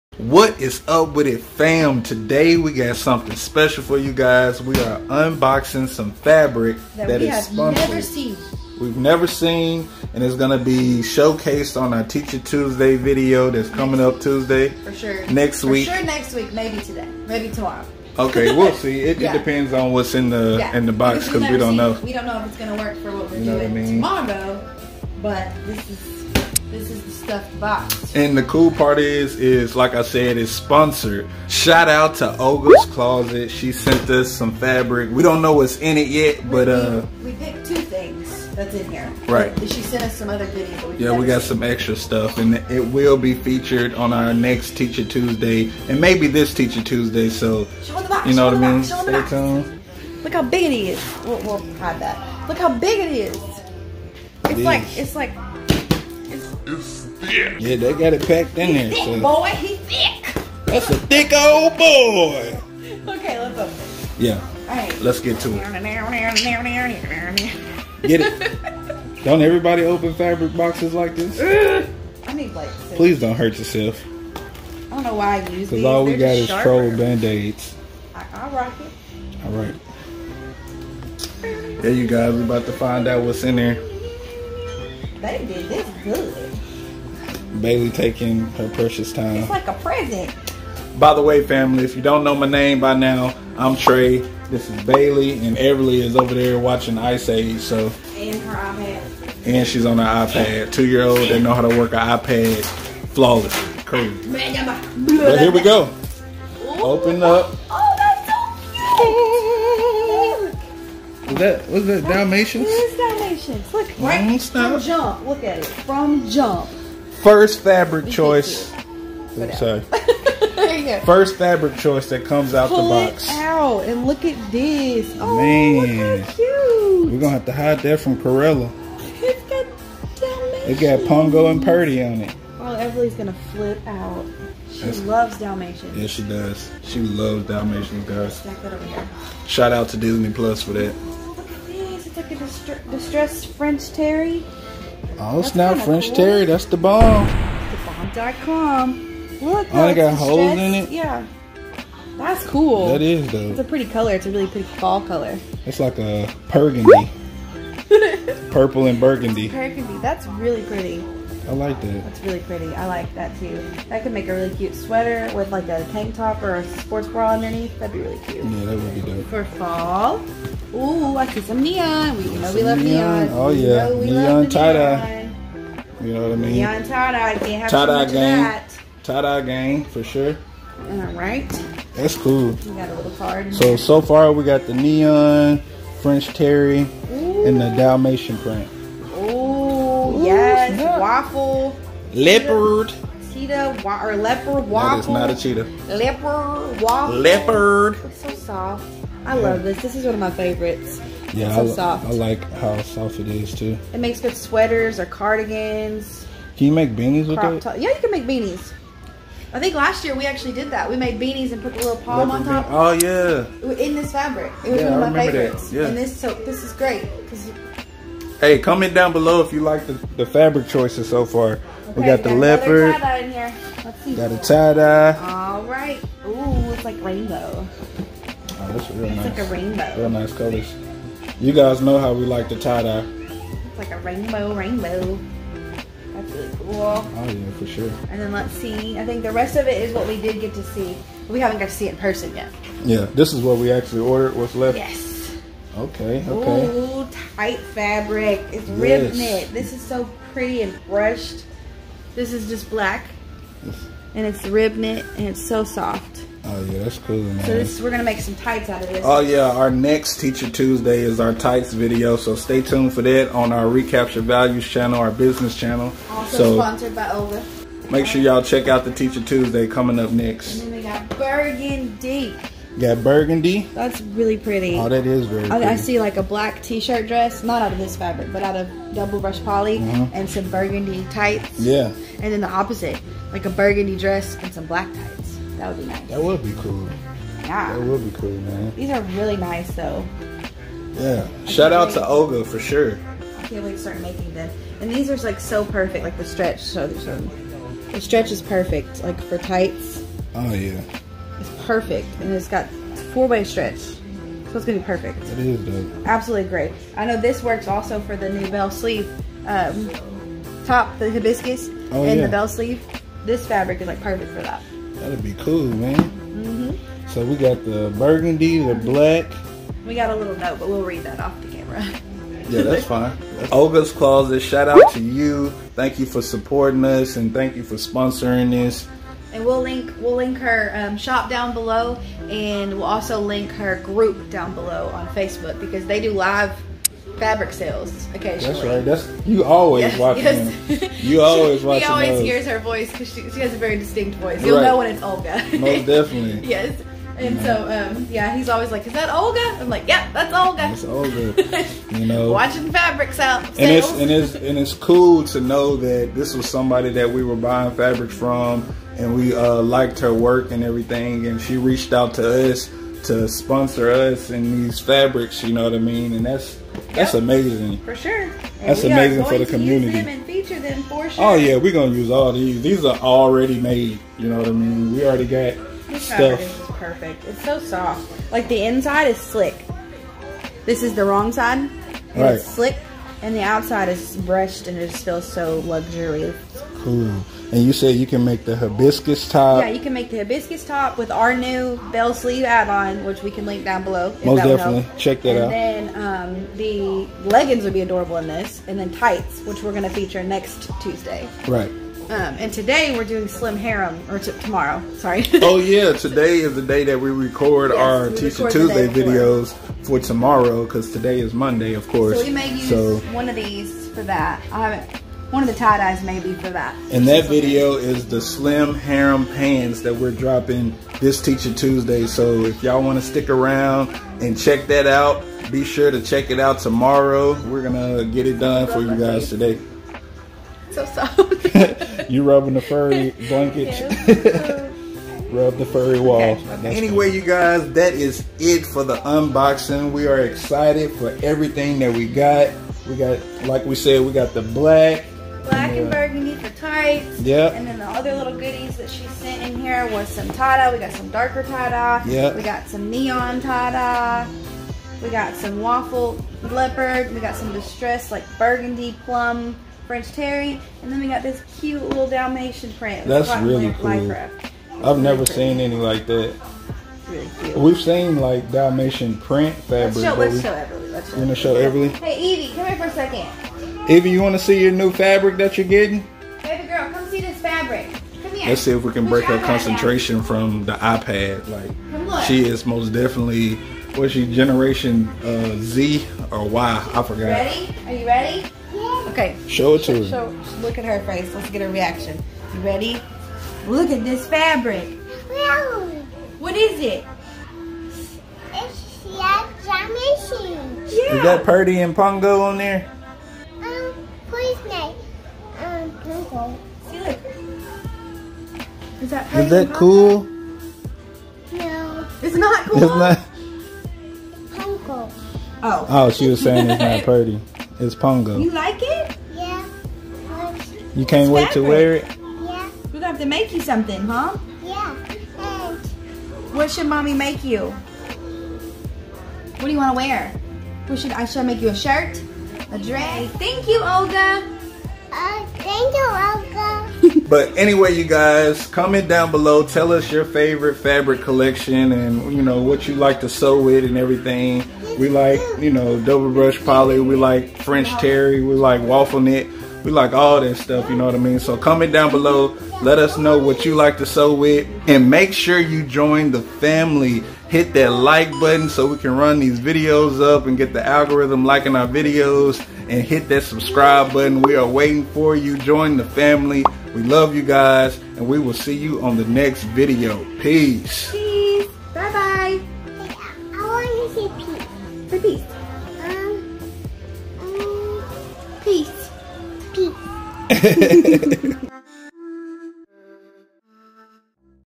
Unboxing Sponsored Fabric | Recaptured Values
Unboxing Sponsored Fabric, Recaptured Values first family unboxing video and we are excited about whats inside! The excitement of not knowing what kind of fabric maybe in a box is one of the best parts of have a Handmade Children's Clothing Business! SHOUT OUT to Olga's Closet who Sponsored the Fabric we received in the box and we hope you all enjoy this video, family business as usual full of love laughter and fabric!